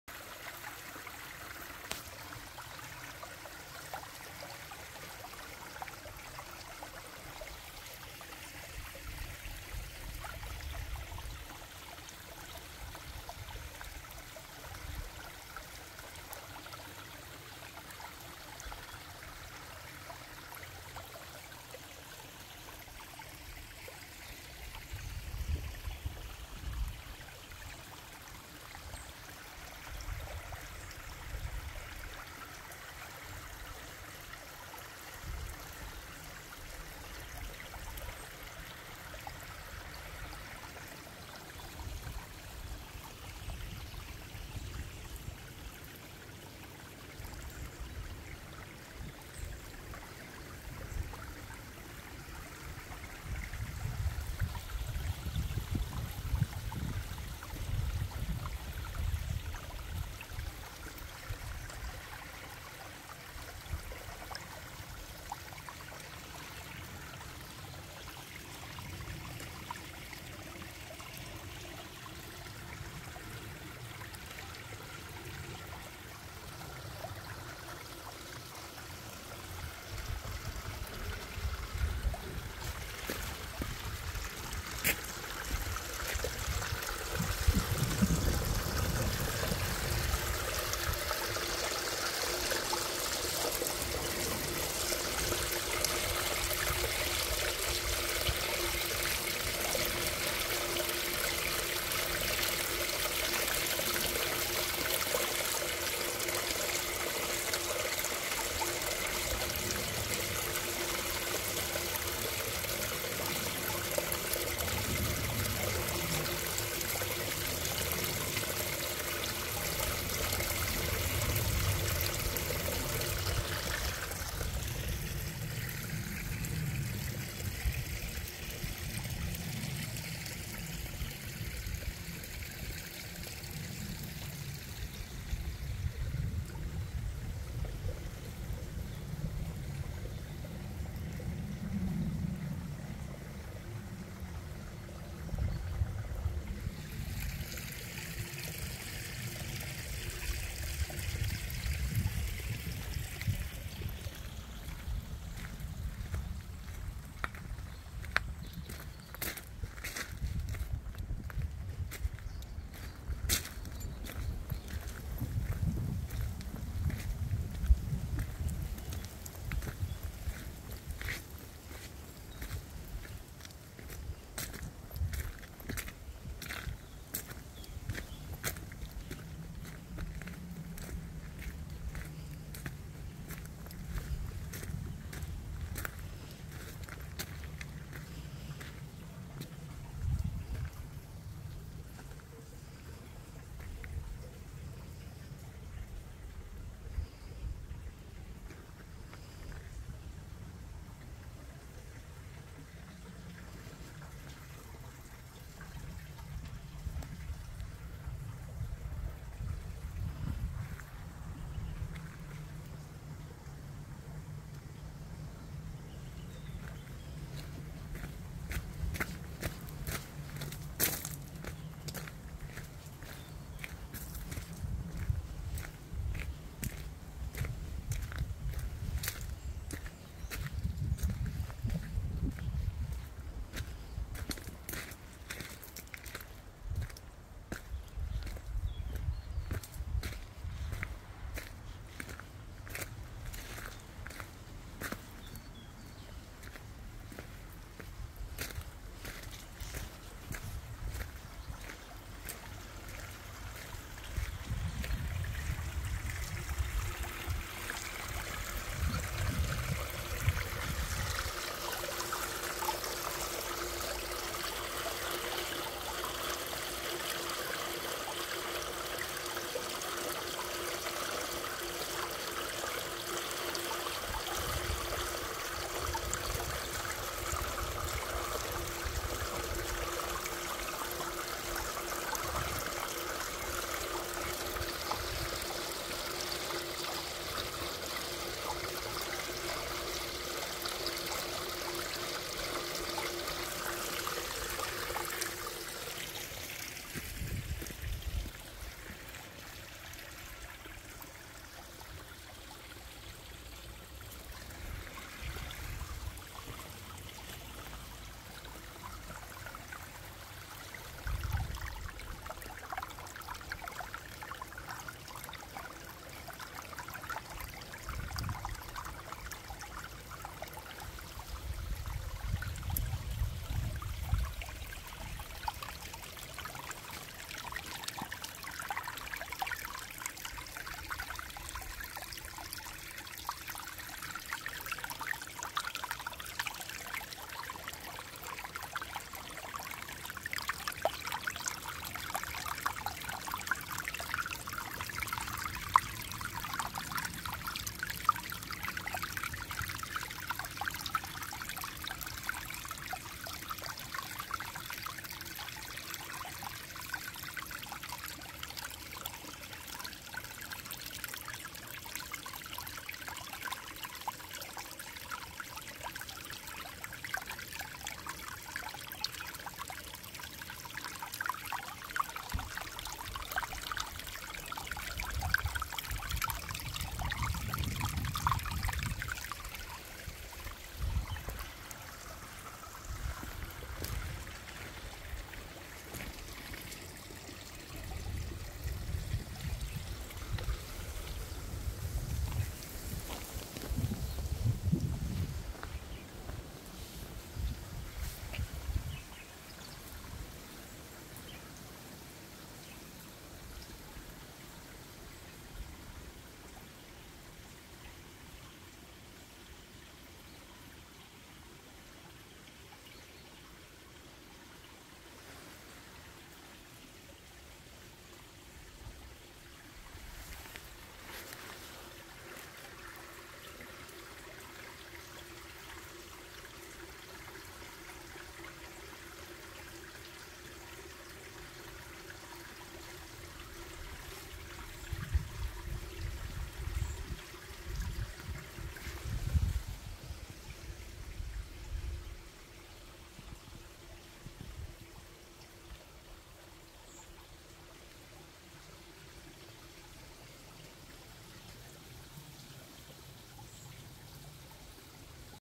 use this to relax😍 very satisfying water sounds asmr and rainy day vibes😊
the perfect rainy day waterfall asmr  to listen to when you're in need of something relaxing😊  A video I filmed last winter during a rainy day with the intention of posting at some point but never got around to doing it. Thanks to ‎@YellowKittyGoblin  I'm back to YouTube after a long time of neglecting the channel and felt like posting something relaxing and very satisfying today  as I am in desperate need of some self care😔 I might have to steal her idea and do a mini self care treatment either today or tomorrow and I might decide to film that and post as well😅  in the Meantime , enjoy listening to the soothing calming sounds of A waterfall and a little bit of rain Along with Seeing nature in all of its beauty😊 I hate getting wet in the rain😑 but I absolutely love the smell of rain and foggy weather😍 experiencing nature flourishing and all of the plants everywhere getting plenty of water So they can thrive😊 plus I love the snails that come out😊   lmk if you'd like to see more asmr type content like this and I'll make efforts to comply with all appropriate suggestions you have😊